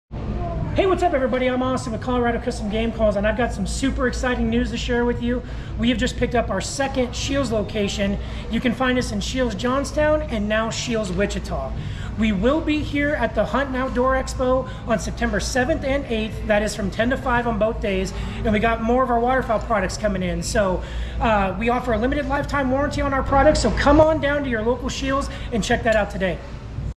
CCGC now in 2nd Scheels location!
We are super excited to announce that we have picked up our second Scheels location now in Wichita